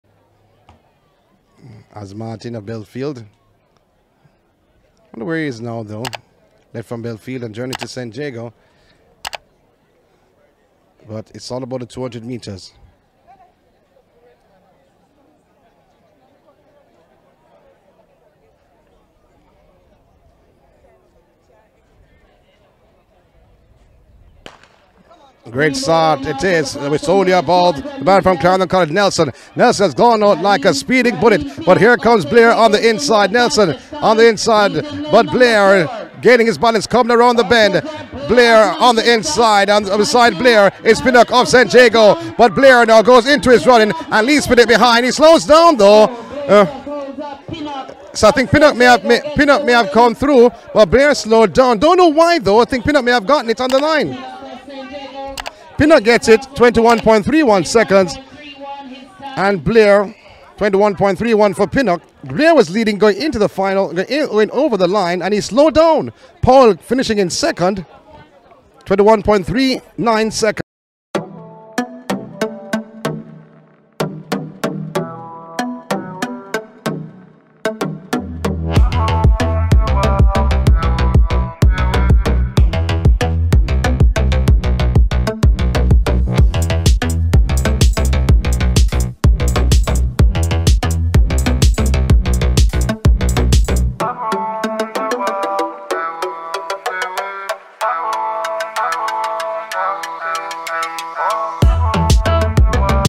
Raheem Pinnock's Impressive Performance in Class 1 Boys' 200m Win - Watch the Race Now!
The Class 1 Boys' 200m event at the Central Champs held at GC Foster College on Thursday, March 2, was an intense competition between some of the most skilled young male athletes in the region. Raheem Pinnock of St. Jago High School secured first place with an impressive time of 21.32 seconds, followed closely by Antonio Powell of Edwin Allen High School in second place with a time of 21.40 seconds, and Javoon Blair of Manchester High School in third place with a time of 21.44 seconds.